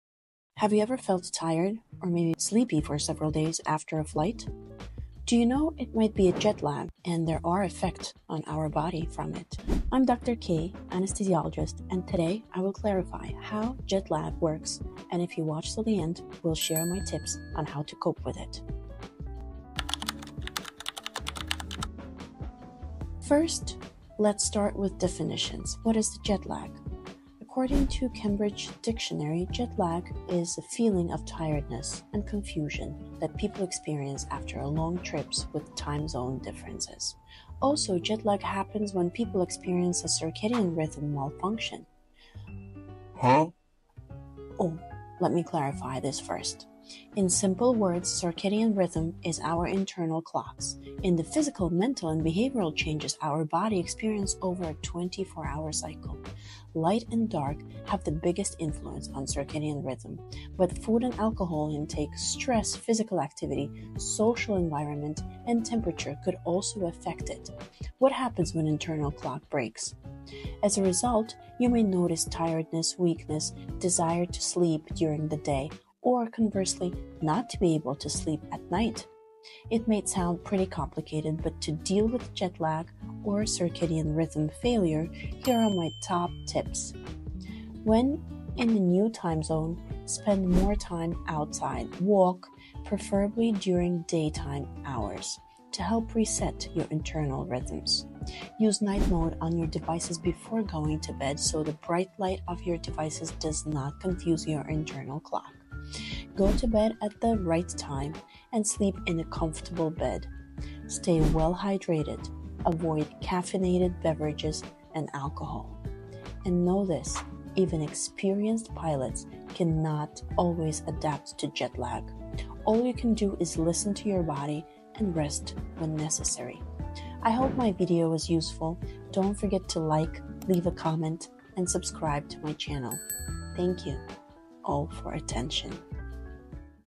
Jet Lag Survival Guide: How Your Body Handles Time Zones
Recently I have travelled with a several time zones differences which triggered this topic for discussion

Let me know how you handled JetLags and what works for you